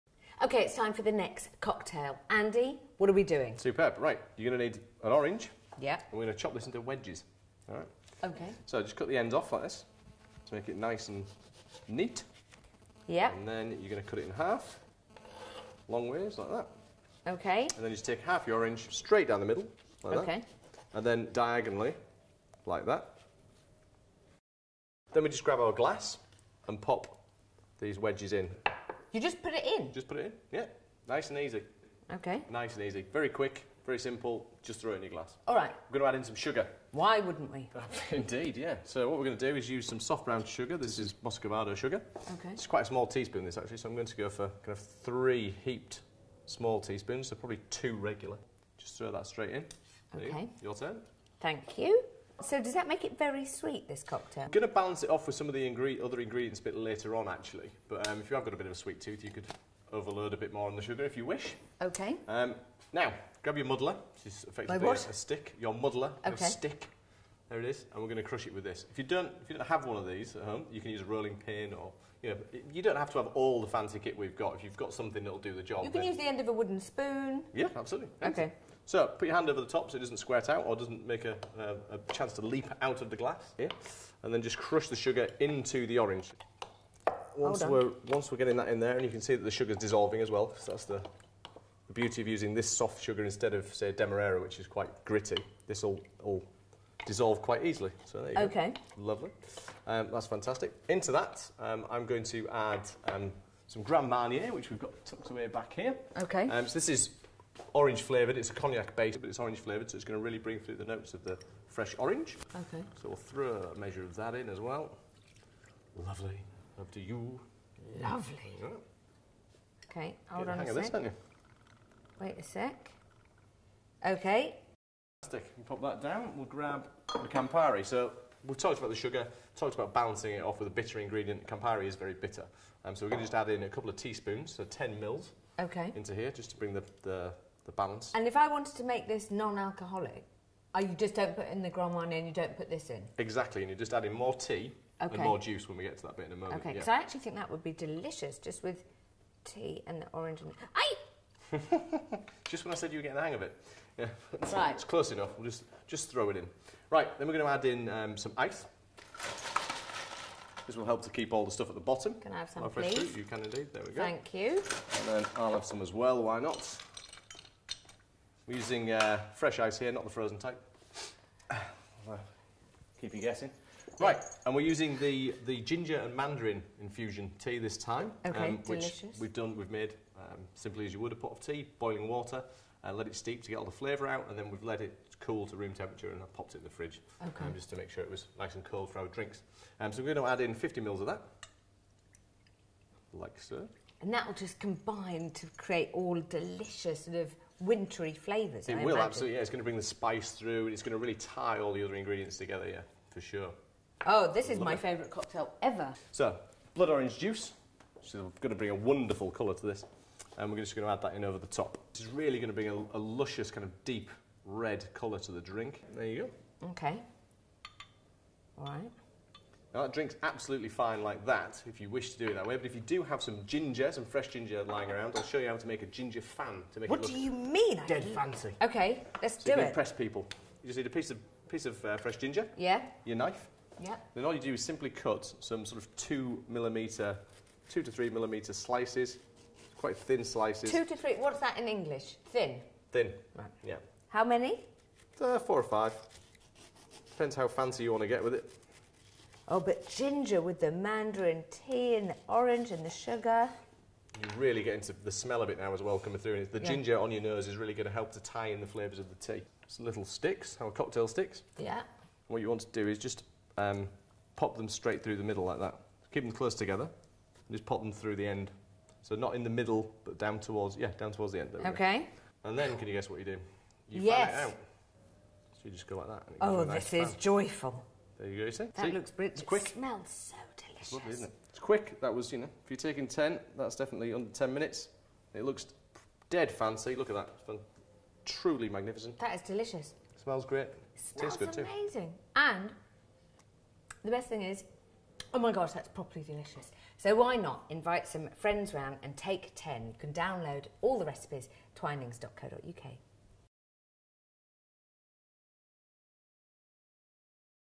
Twinings - Cocktails with Claudia Winkleman - Ginger & Mandarin
Ginger & Mandarin Cocktail Cocktail:  6 Orange wedges muddled with 2 large teaspoons of soft brown sugar (or moscovado sugar), 35ml Grand Marnier, 10ml Campari, 75ml Twinnings Revive Organic Ginger & Manderine Tea (with added caster sugar), 50ml Blood Orange Juice (Tropicana Sanguinello Juice works),
Prepared directly into a rocks glass, with a fresh ginger fan garnish.